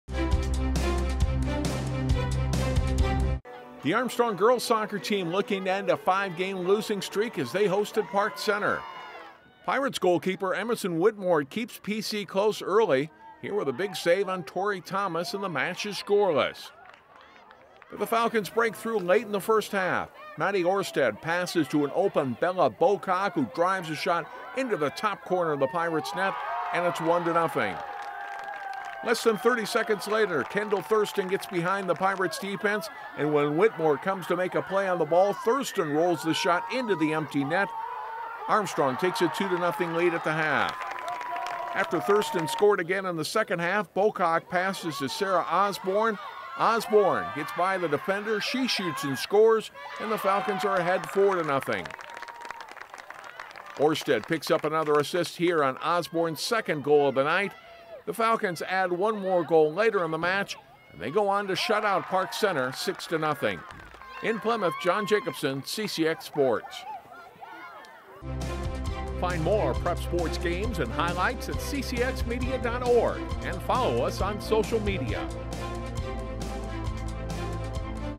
Armstrong Girls Soccer Blanks Park Center
Sarah Osborne and Kendall Thurston scored two goals each leading the Armstrong girls soccer team to a 6-0 win over Park Center Tuesday. The Falcons started the season with a win and a tie but had lost five straight games since. The six goals were a season high for Armstrong (2-6-1). The Falcons play at Northwest Suburban Conference co-leader Champlin Park Thursday while Park Center (0-9-0) hosts Andover.